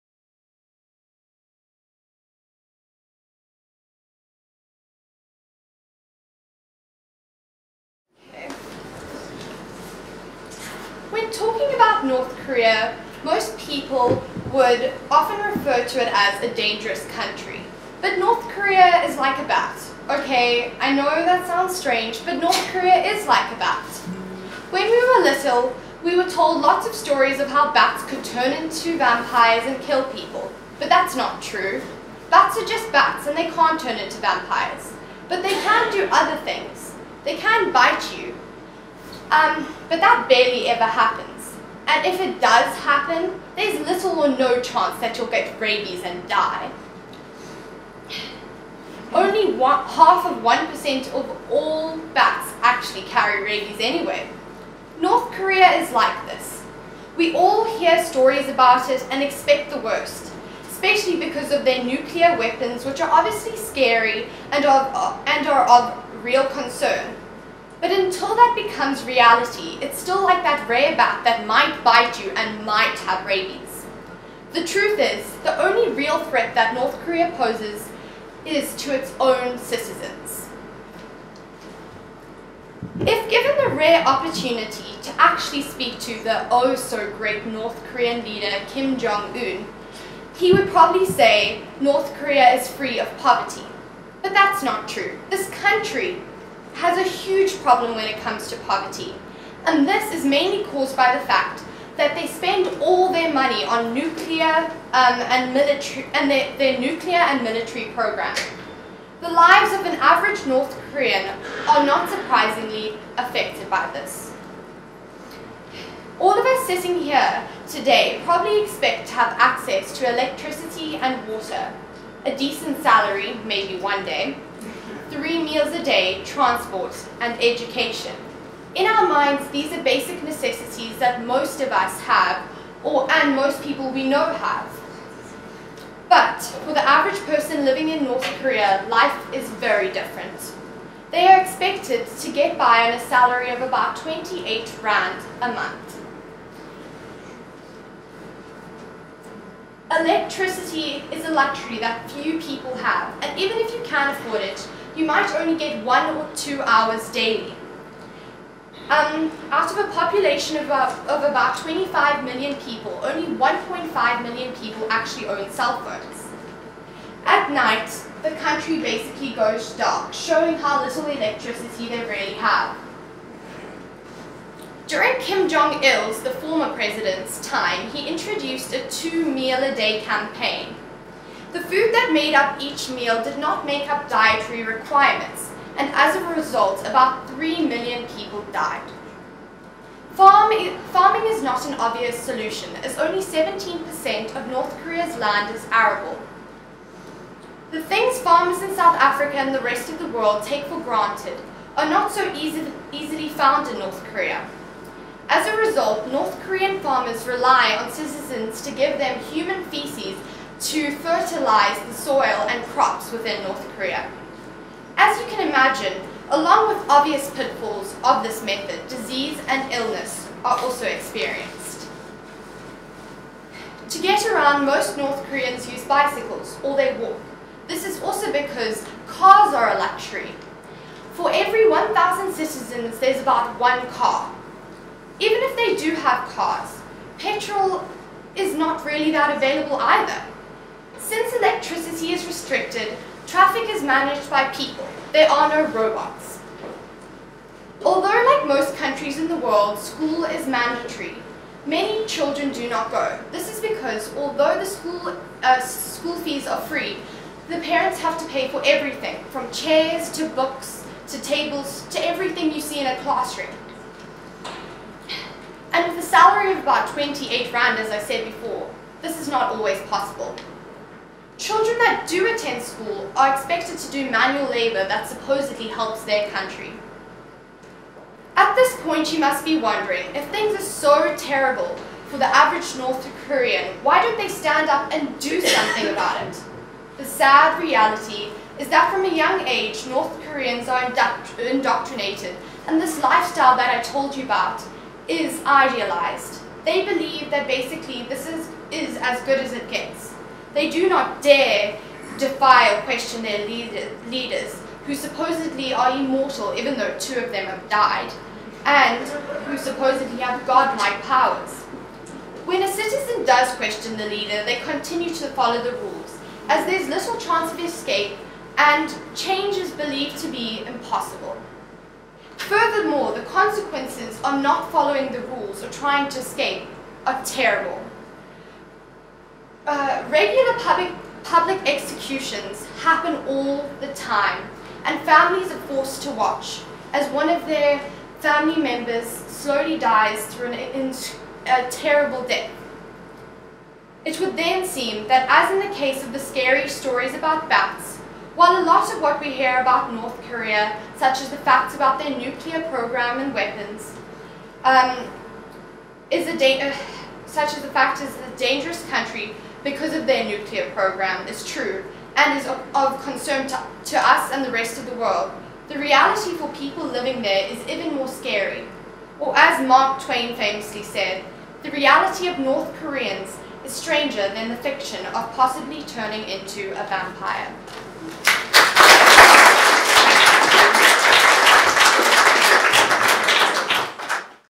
TED Ed Club - Caitlin Murphy: North Korea: The people's story
Caitlin was part of the first TED-Ed Club in South Africa - presented by Think Ahead - and this is her talk on North Korea.
The TED-Ed Club program supports students in discovering, exploring and presenting their big ideas in the form of short, TED-style talks.
Caitlin is a student at Roedean Girls' School in Johannesburg.